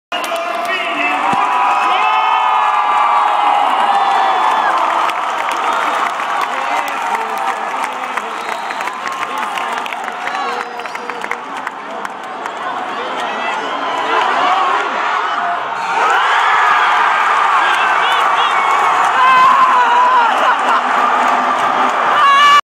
Indiana State's Jake Kelly hits shot to beat Missouri State
Jake Kelly hits a lay up and gets fouled with .6 left in the 2nd half to tie MSU 69-69.